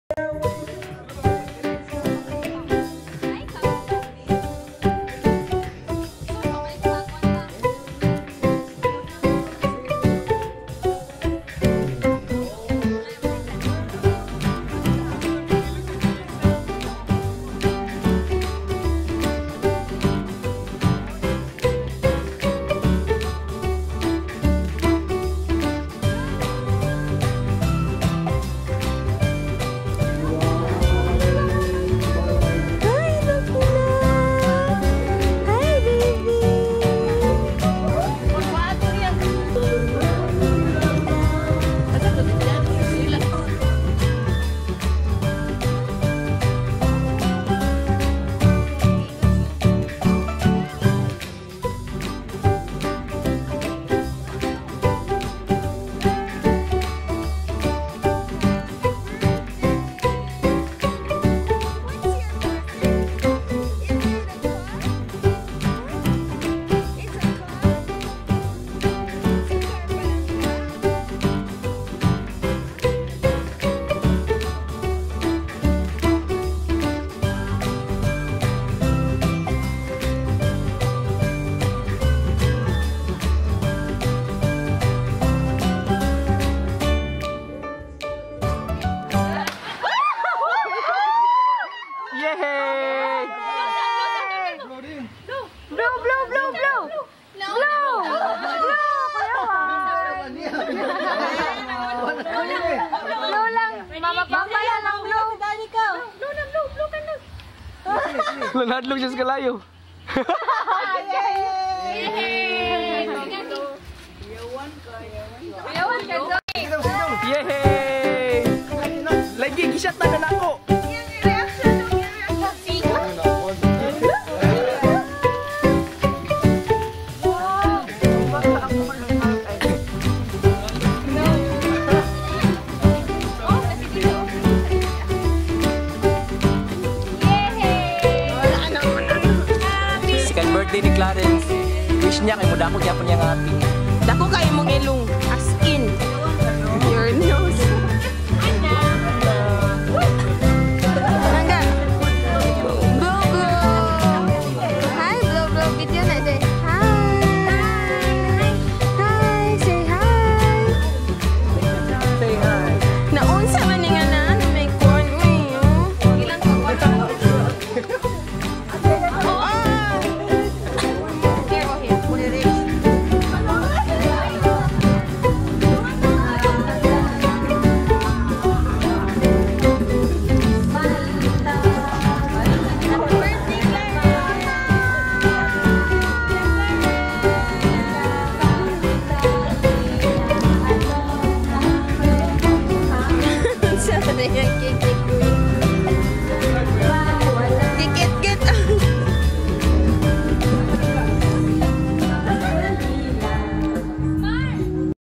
Clarence's 2nd birthday
Clarence second birthday at ypil's pool at Buru-un, Iligan City last August 10, 2015.